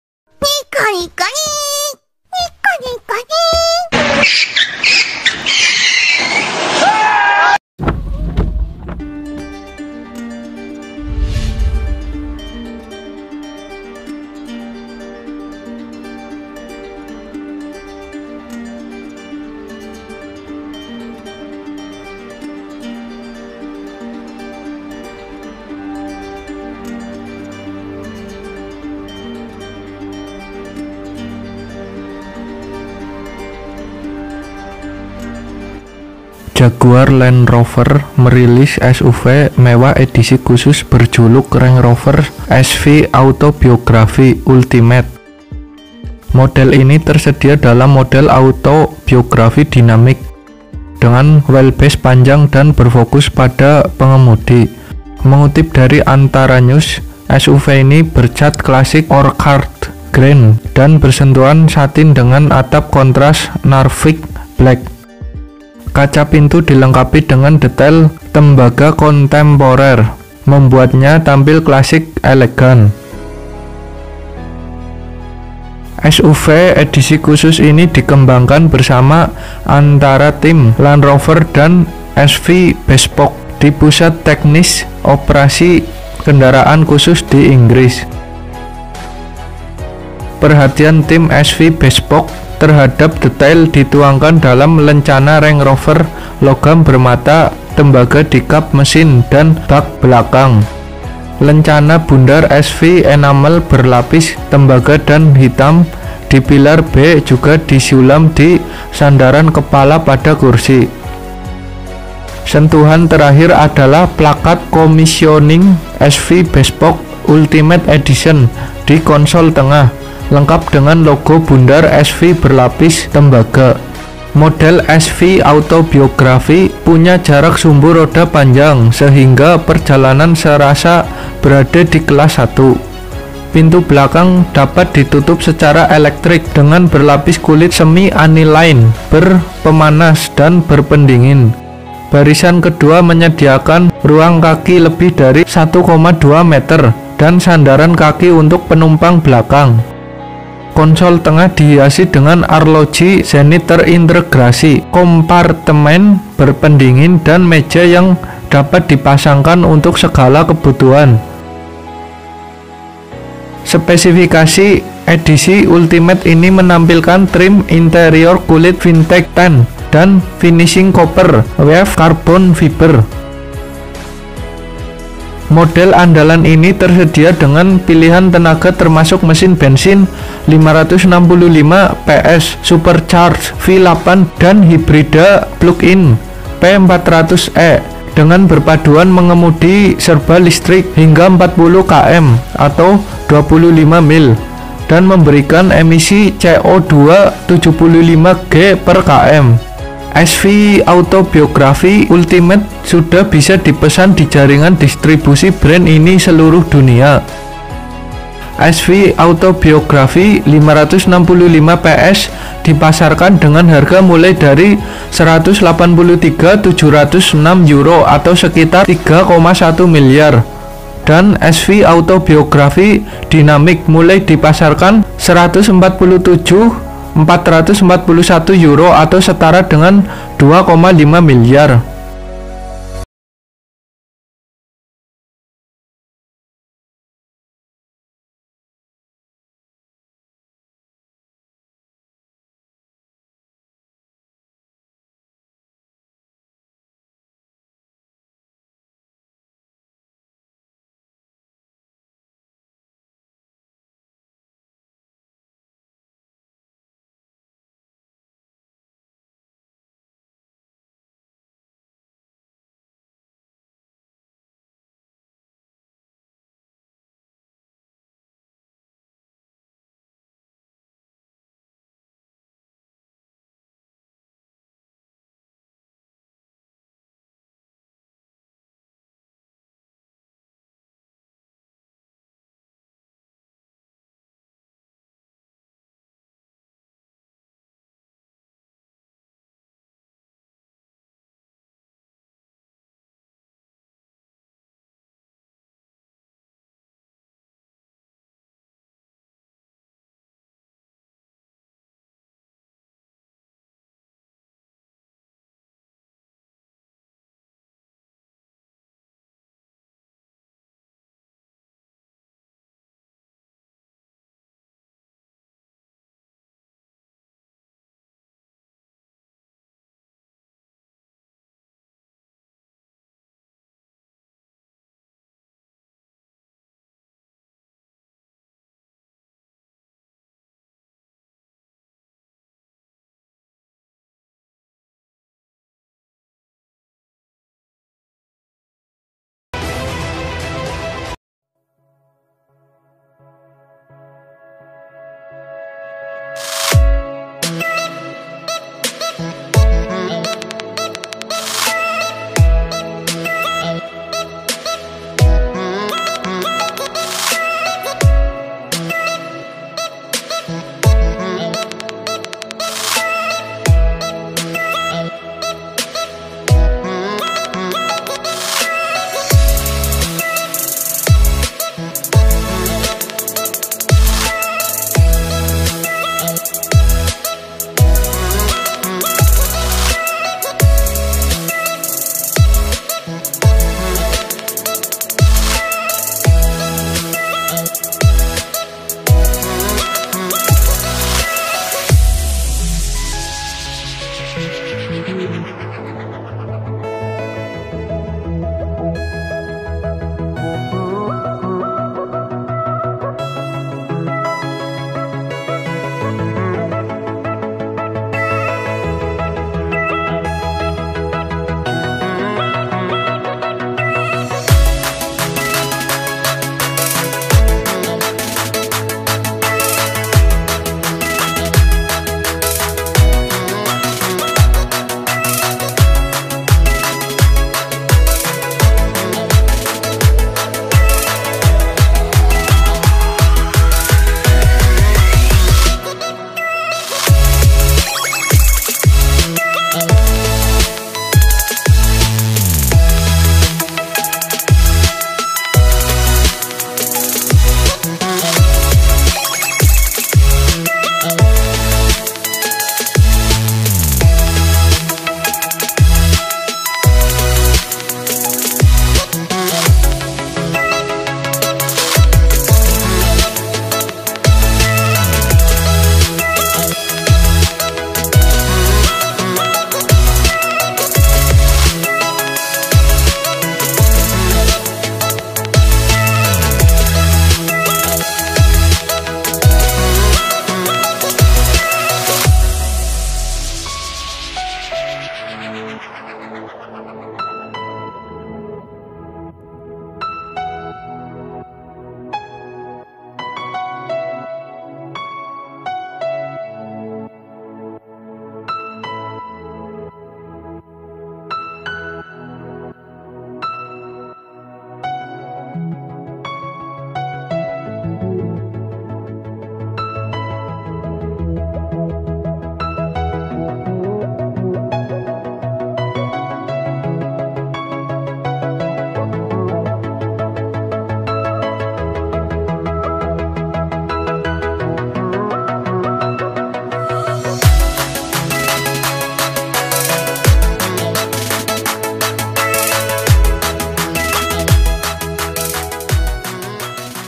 RANGE ROVER AUTOBIOGRAPGHY 2022 REVIEW || Jaguar Eropa terbaru hadir dalam varian 7 seat
Jaguar Land Rover merilis SUV mewah edisi khusus berjuluk Range Rover SVAutobiography Ultimate. Model ini tersedia dalam model SVAutobiography Dynamic dengan wheelbase panjang dan berfokus pada pengemudi.

Mengutip Antaranews, Rabu (7/4/2021), SUV ini bercat klasik Orchard Green dan sentuhan satin dengan atap kontras Narvik Black. Kaca pintu dilengkapi dengan detail tembaga kontemporer, membuatnya tampil klasik elegan. SUV edisi khusus ini dikembangkan bersama antara tim Land Rover dan SV Bespoke di Pusat Teknis Operasi Kendaraan Khusus di Inggris.

SVAutobiography Ultimate sudah bisa dipesan di jaringan distribusi brand ini seluruh dunia. SVAutobiography 565PS dipasarkan dengan harga mulai 183.706 euro atau sekira Rp3,1 miliar dan SVAutobiography Dinamic mulai 147.441 euro atau setara Rp2,5 miliar.